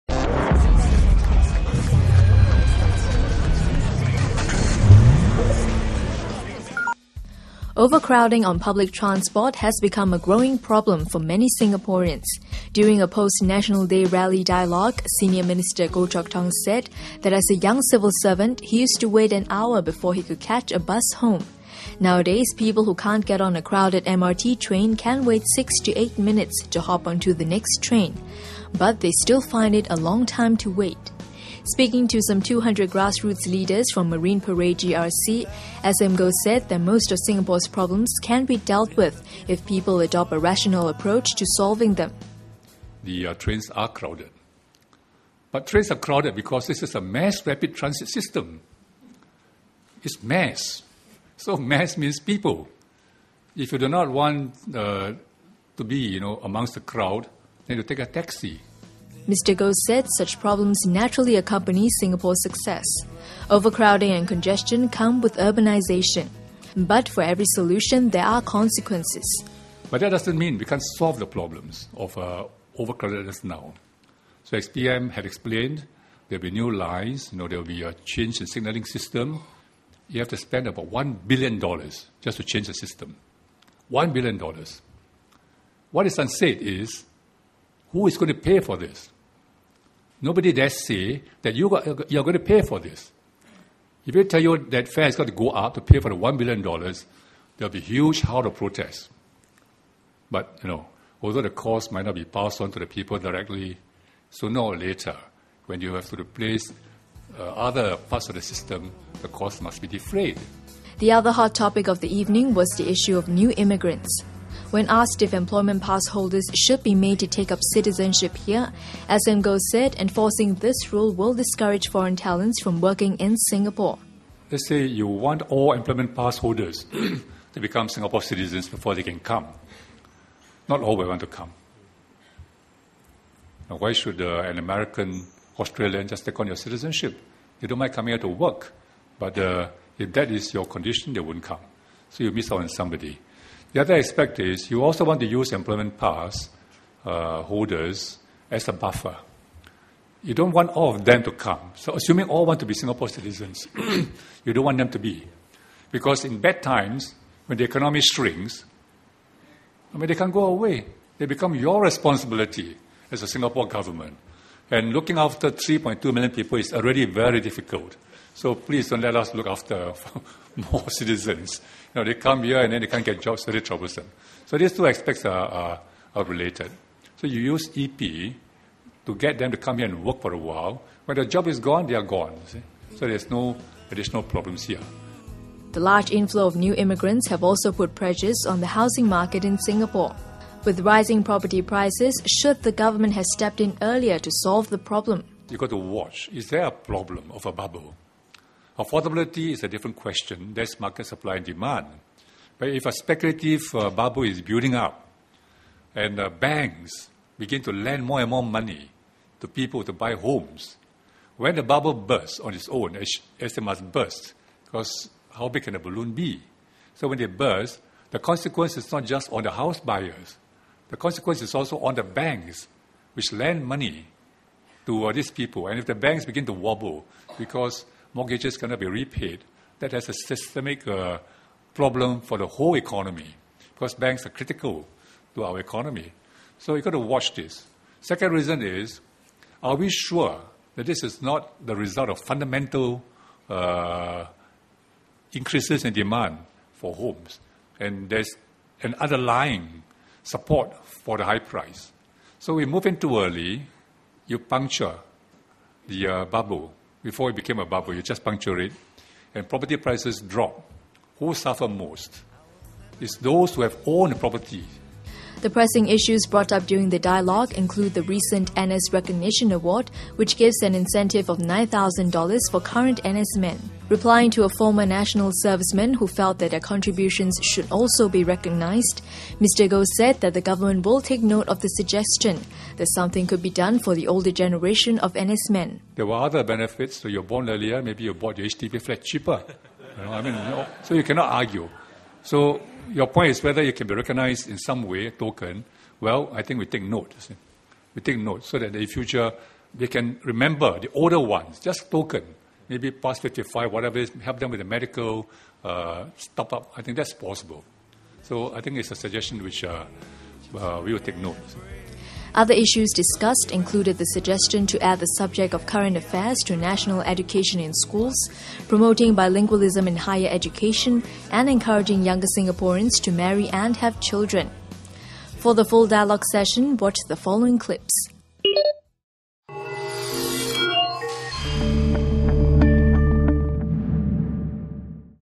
Overcrowding in trains can be resolved (Dialogue with SM Goh Pt 1)
SM Goh answered transport concerns at a dialogue session at the Marine Parade GRC.